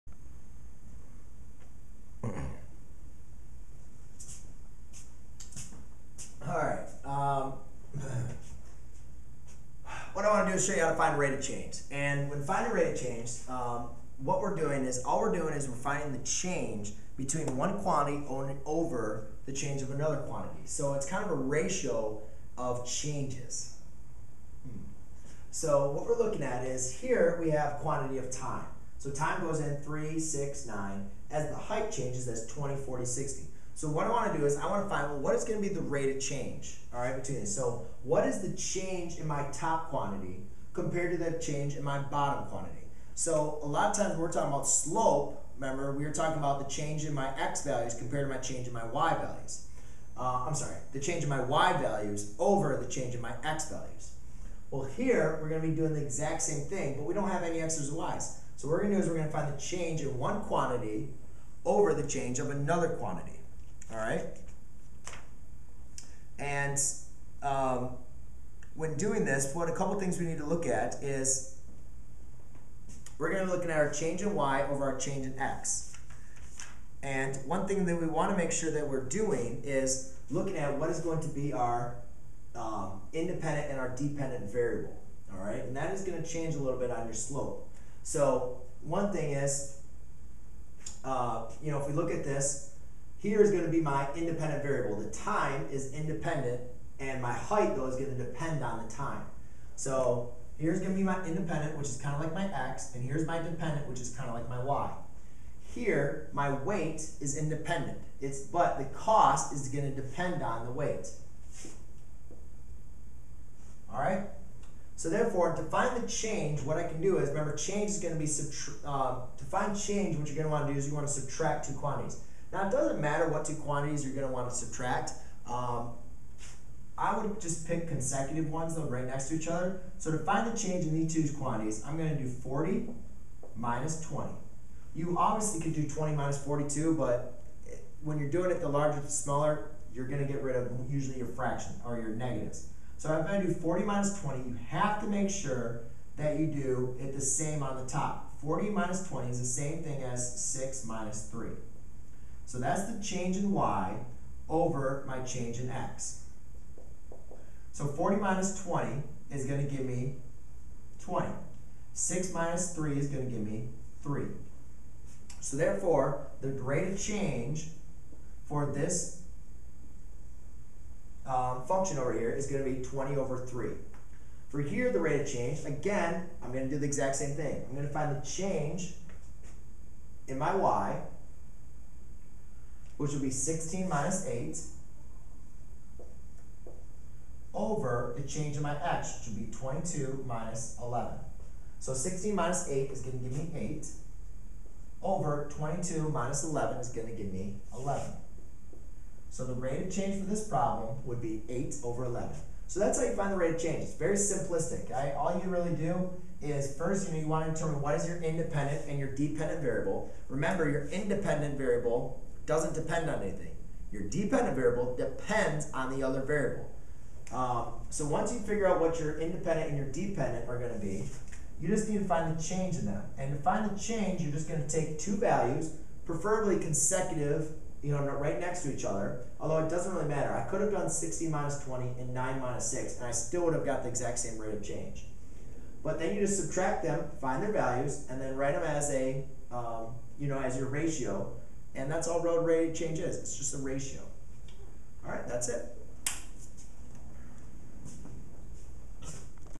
Find the rate of Change given a table
👉 Learn how to find the rate of change from a table of values. The rate of change of a set of data listed in a table of values is the rate with which the y-values are changing with respect to the x-values. To find the rate of change from a table of values we determine the rate at which the y-values are changing and divide it with the rate at which the x-values are changing. i.e. rate of change = change in y / change in x.